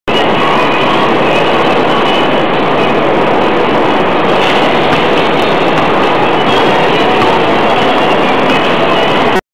Freshview Cinema at Manda Hill Lusaka
Freshview Cinema opened Friday 2nd Dec 2011 in Lusaka at Manda Hill mall.